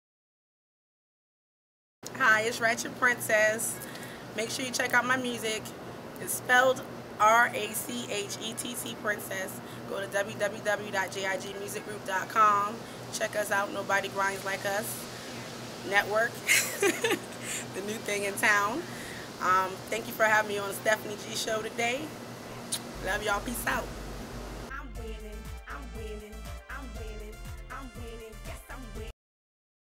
Rachett Princess commercial
Rachett Princess talks about her new project and where you can hear her music!

PD the CEO